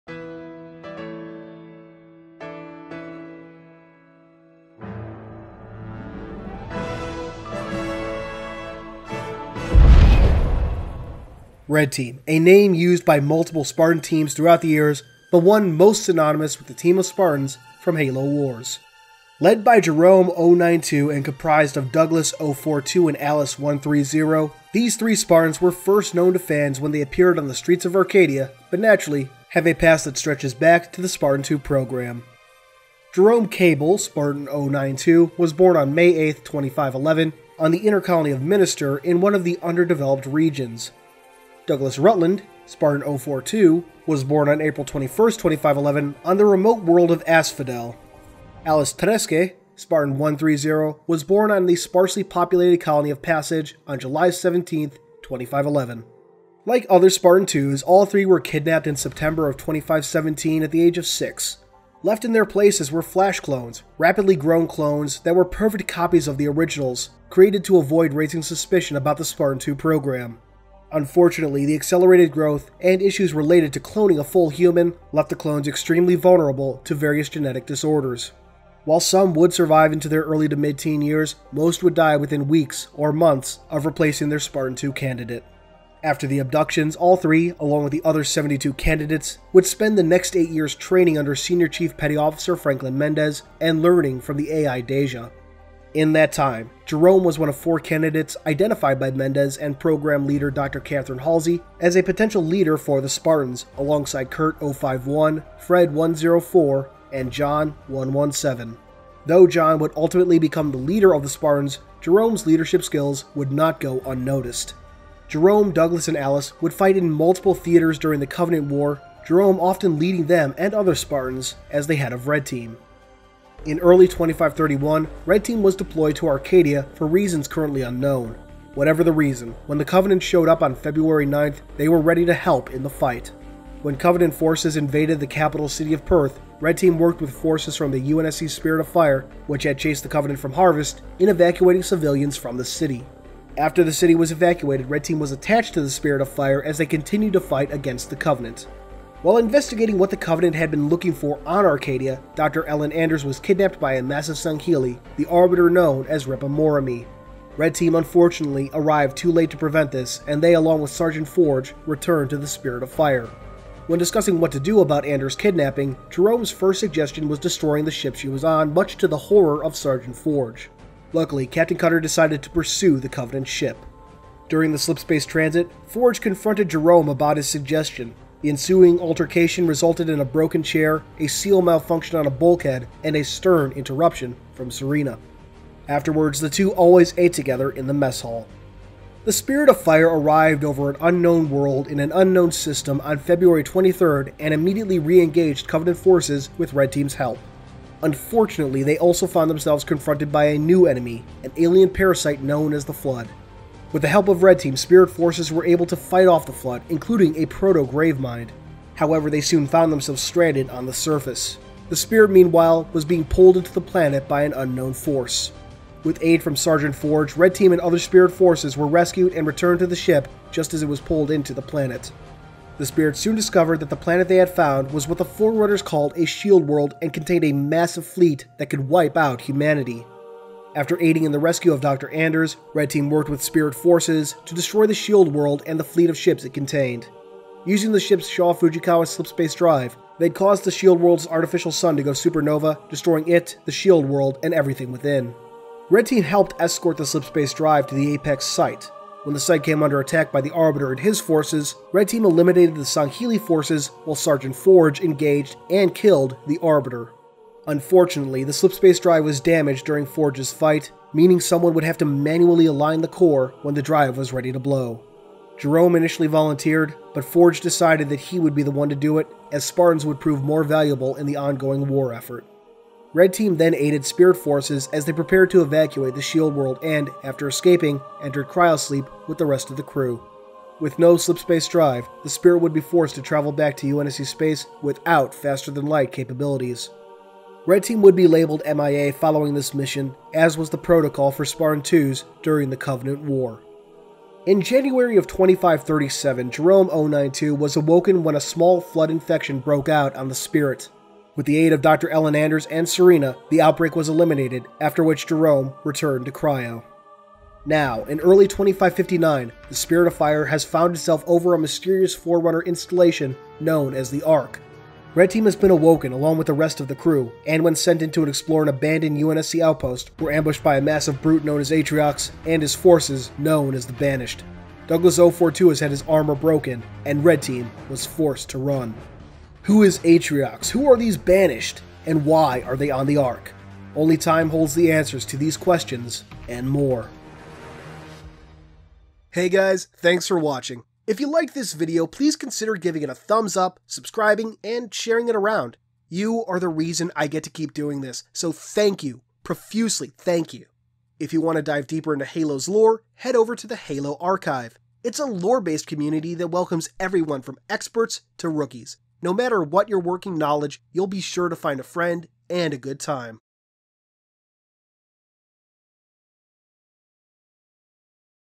History of Red Team - Halo Wars 2 Primer Series
Red Team, a name used by several groups of Spartans over the years, but one most synonymous with the group from Halo Wars. Today, we discuss the histories of Jerome-092, Douglas-042, and Alice-130.